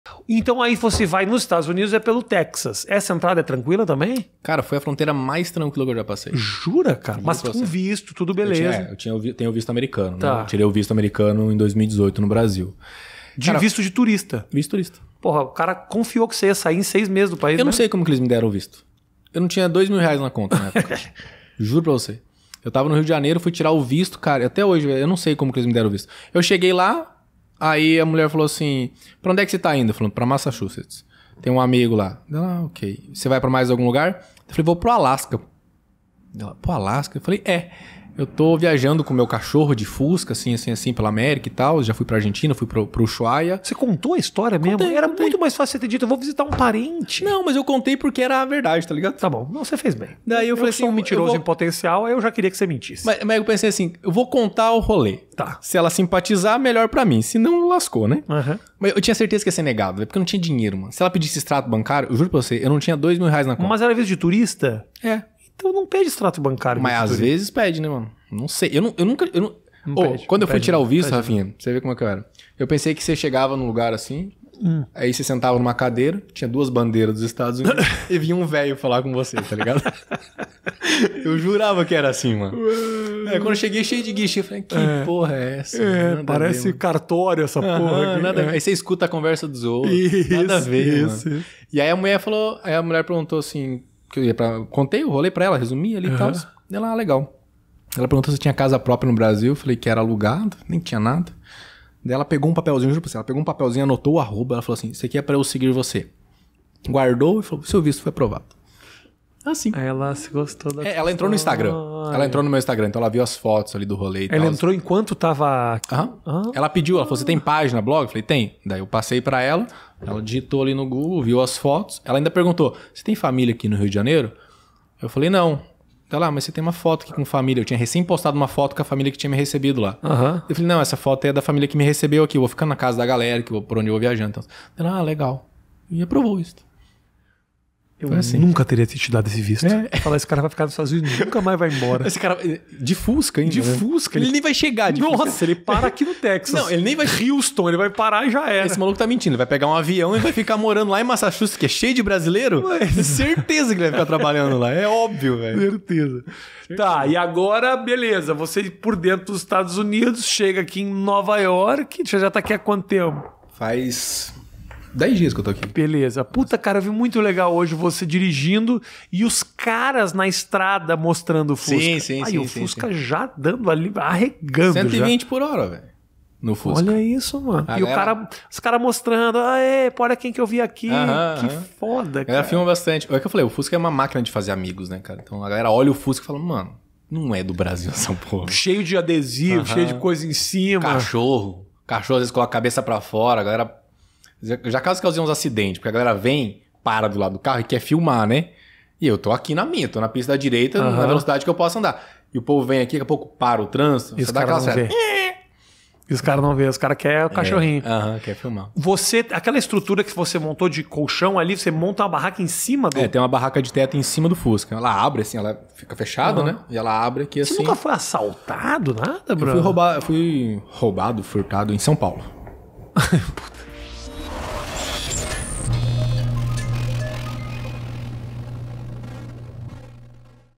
COMO FOI CRUZAR A FRONTEIRAS MÉXICO/ESTADOS UNIDOS - JESSE E SHURASTEY | Cortes Mais que 8 Minutos
Viajar de fusca do Ushuaia até o Alaska, já pensou? É isso que o @Shurastey  tá fazendo! E por sorte ele passou por Nova York e conseguiu bater esse papo nos contando tudo sobre a viagem, que eu tenho certeza que você vai gostar e se inspirar.

Conheça também a lojinha dos dois: @shurastey.dogs
E o perfil pessoal do Jesse: @jessekoz

Host: Rafinha Bastos @rafinhabastos
Edição: Dreiky @doreiki
Artes: Camilla Lourero @lorer0